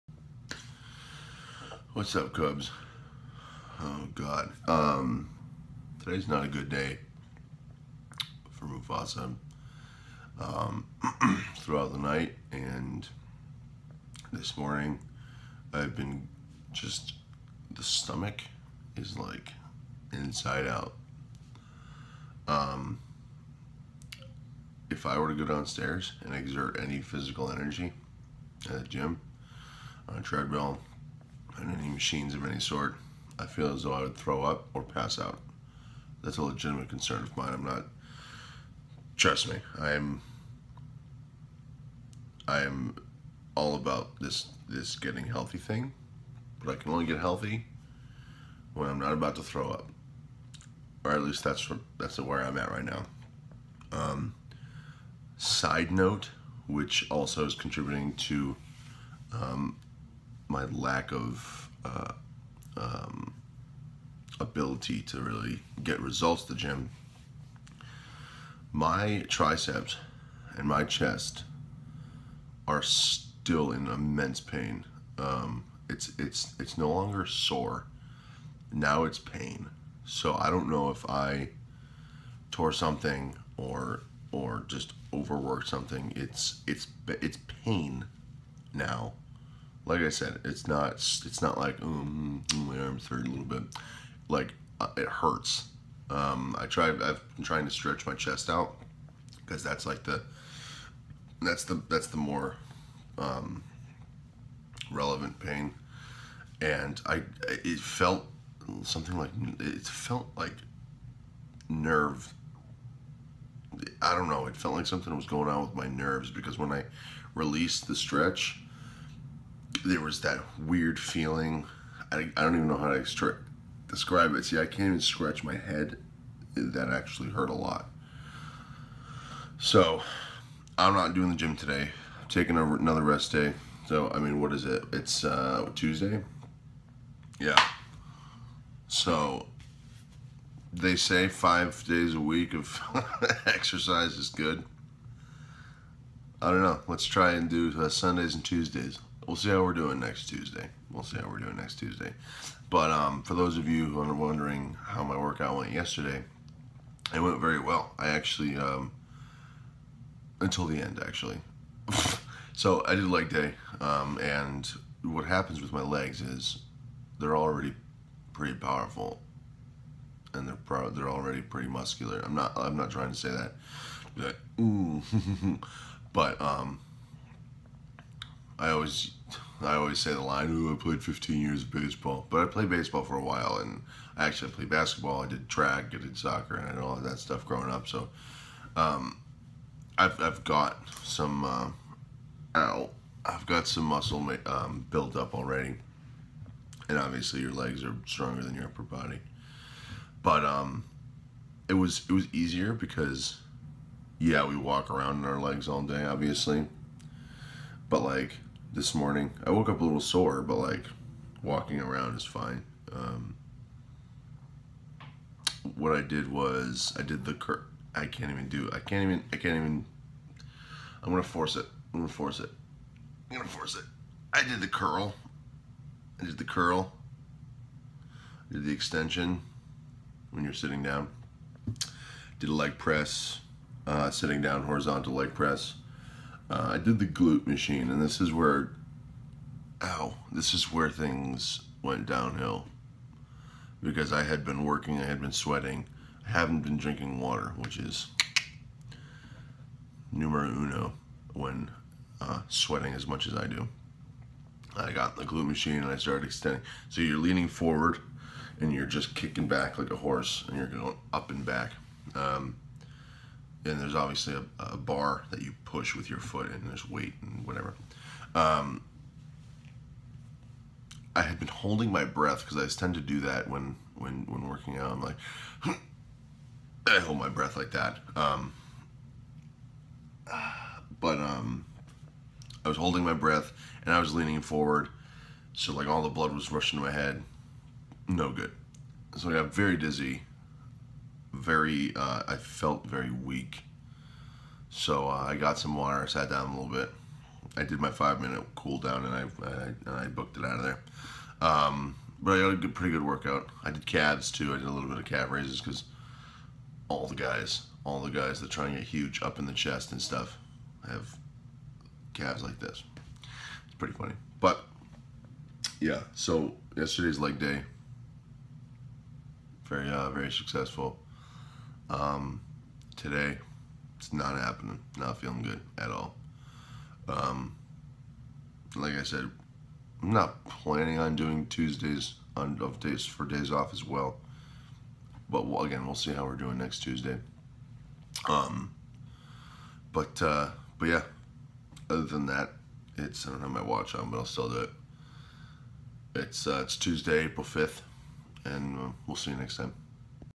MUFASA VS. STOMACH --[5/365]
Woke up this morning wanting to rip my stomach out of my body. Not a good day, folks. But…we'll get em next time!